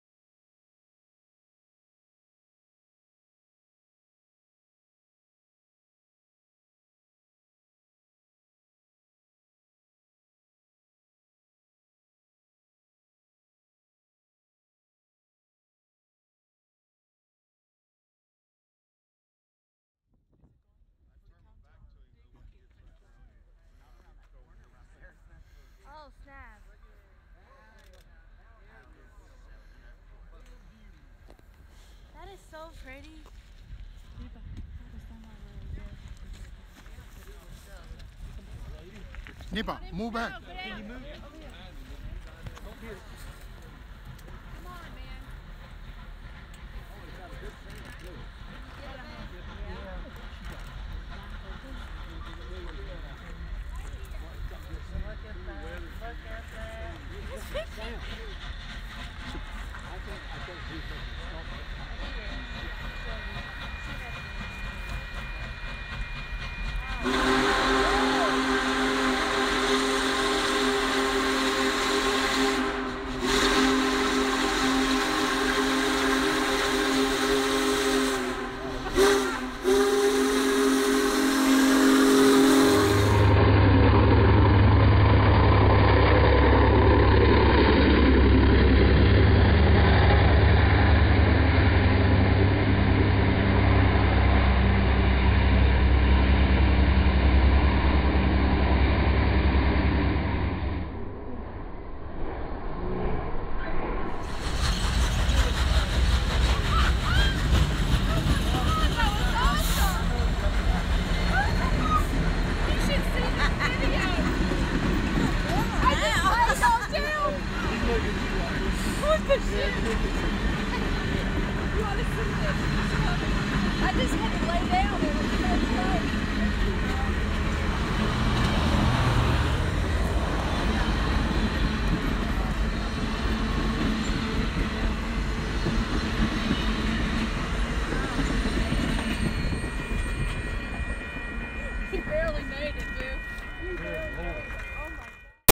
Union Pacific's "Big Boy" 4014
Union Pacific's "Big Boy" going through Jefferson, TX - Nov. 12, 2019